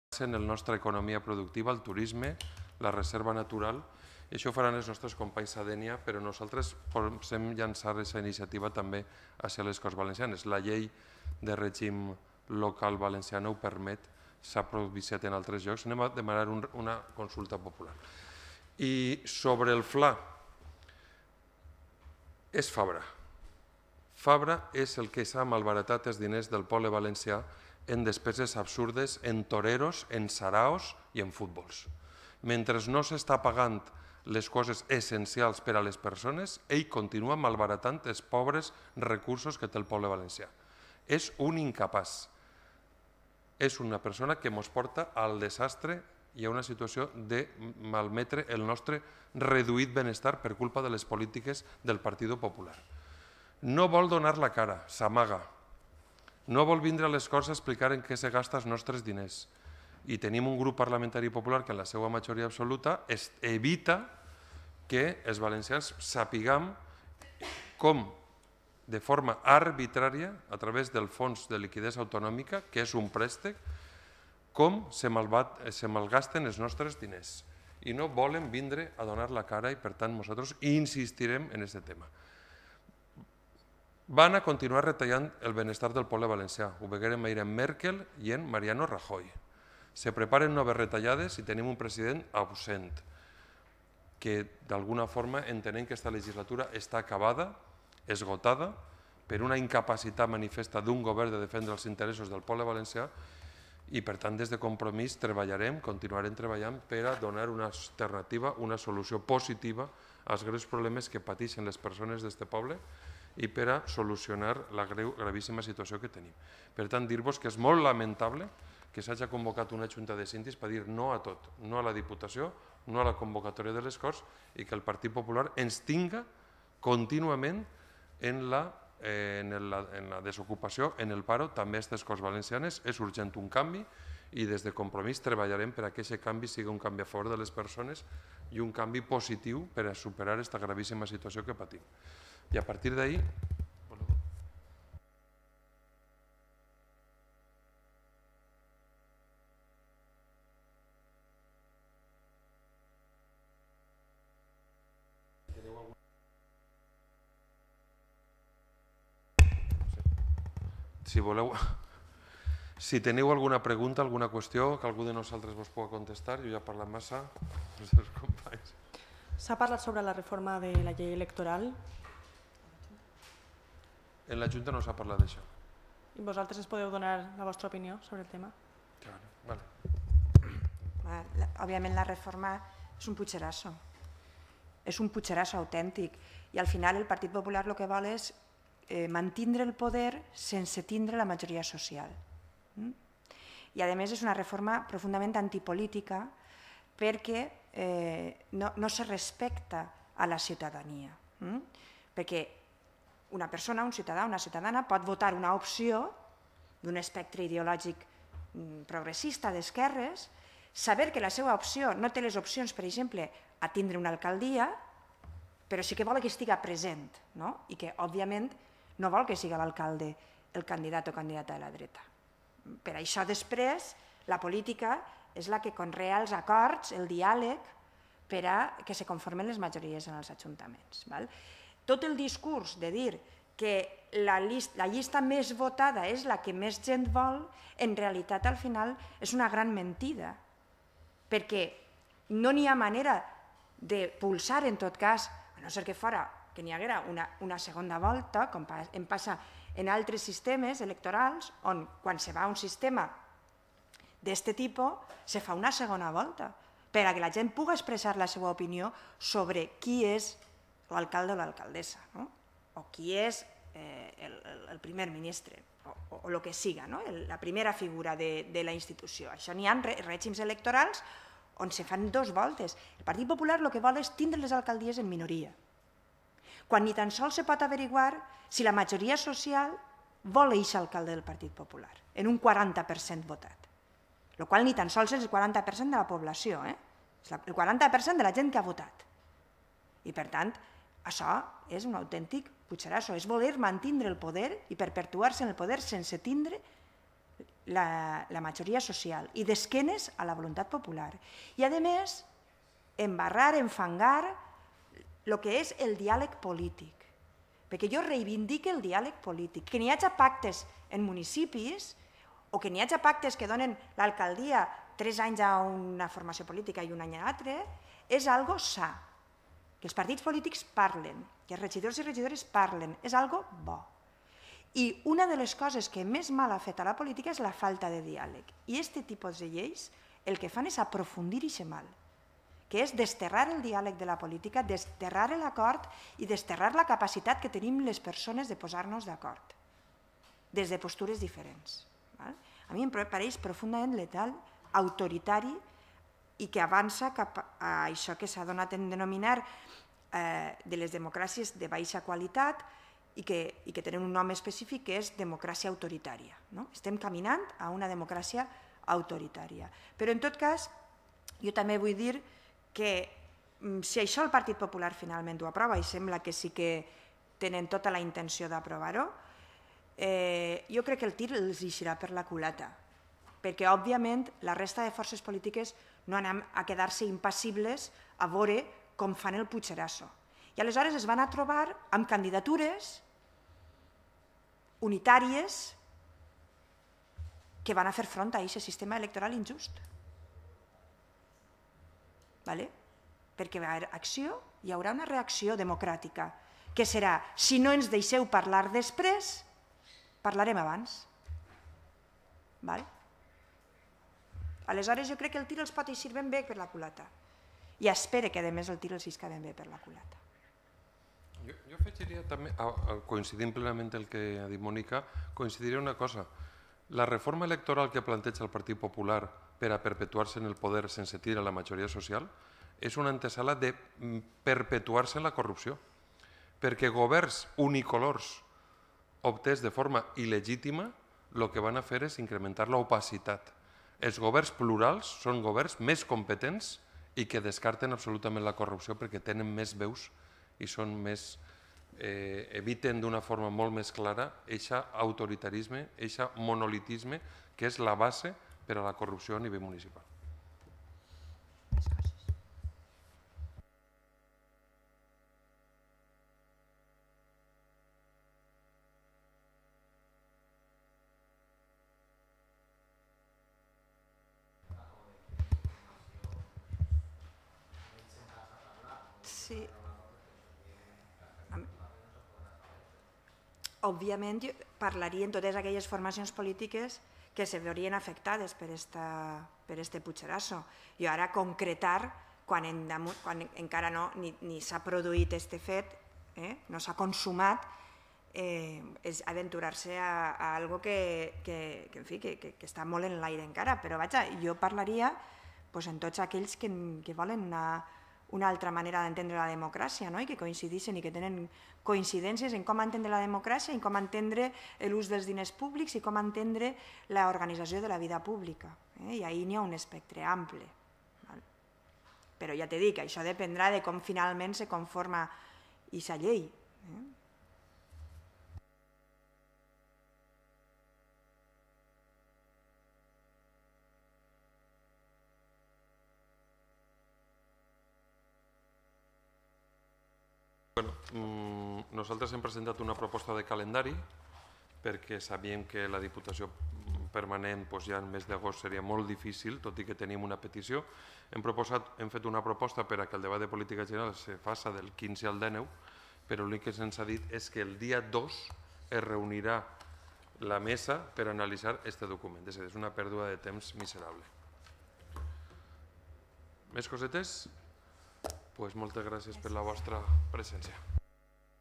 Roda de premsa inici del curs polític 2014-2015
Enric Morera, Mònica Oltra i Fran Ferri compareixen en roda de premsa, després de la junta de síndic celebrada en les Corts, en el PP ha rebutjat totes les propostes de treball presentades per l'oposició.